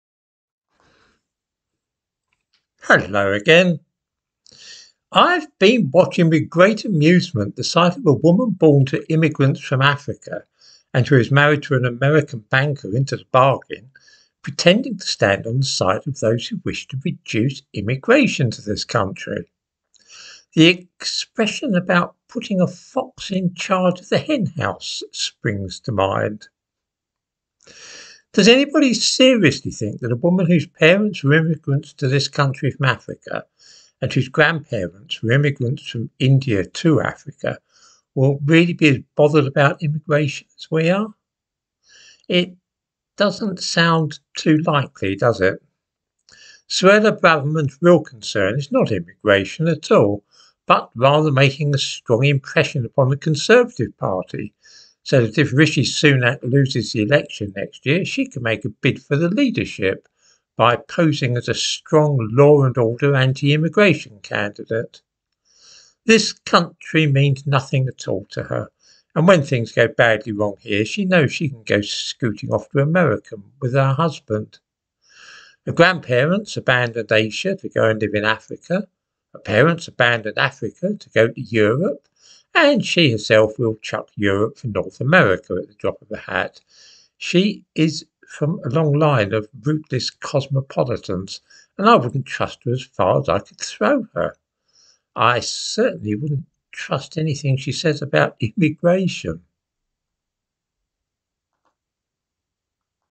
Home Secretary Suella Braverman has no intention at all of halting illegal immigration to Britain
For all her tough talk, Braverman is part of the problem, rather than the solution.